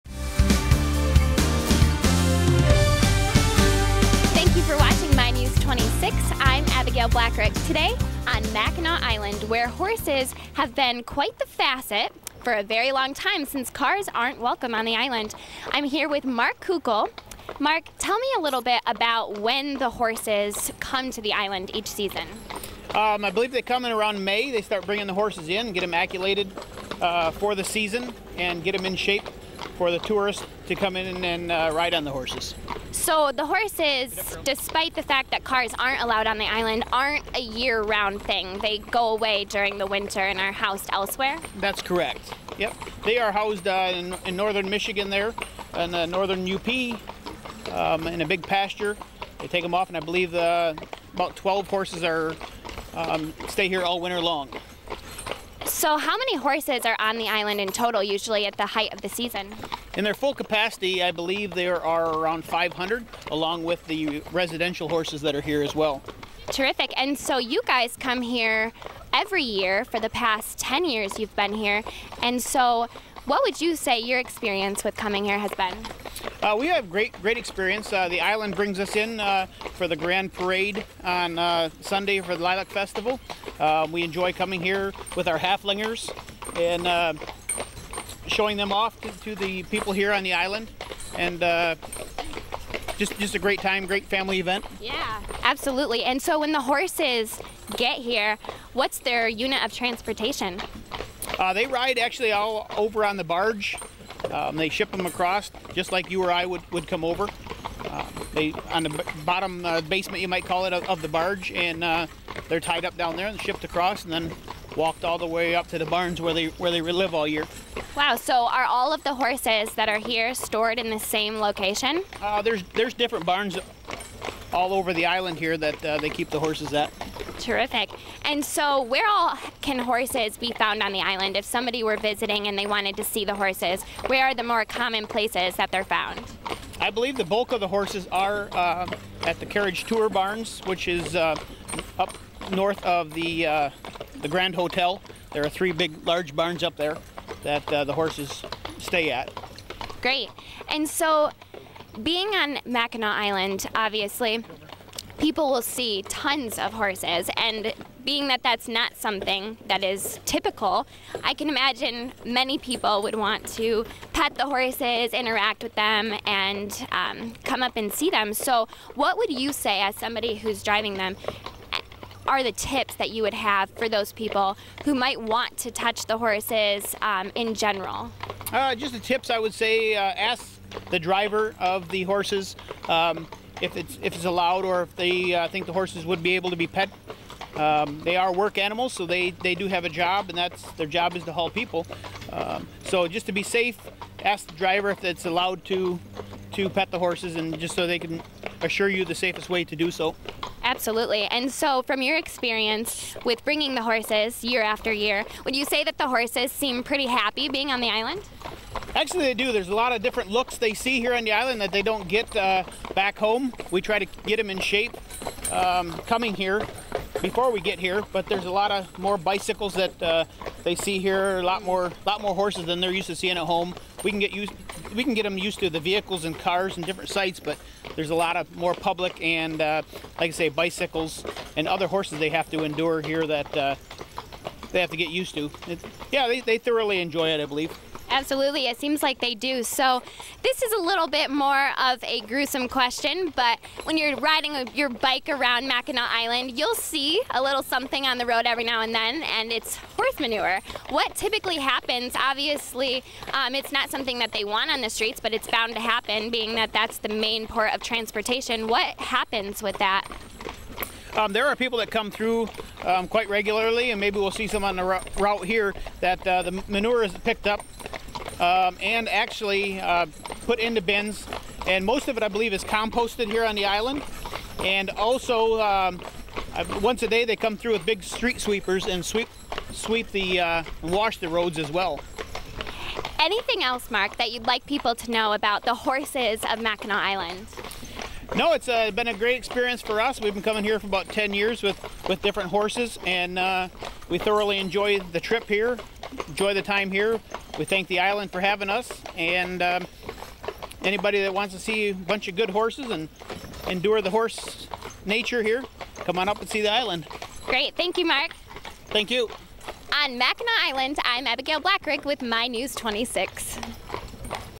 Island Carriages
The island horse carriages have been a major part of the island that tourists are always excited to see and ride. Around 500 horses come in by boat during May, and workers gets them fit and ready for the tourists each year. While the horses are on the island, they are stored in barns all around the island, but the more common places to find them are at the three carriage tours barns north of the Grand Hotel. It's referred for people to ask the drivers if you were wanting to pet the horses.

To find out more details, please contact Abigail Blackrick at MI NEWS 26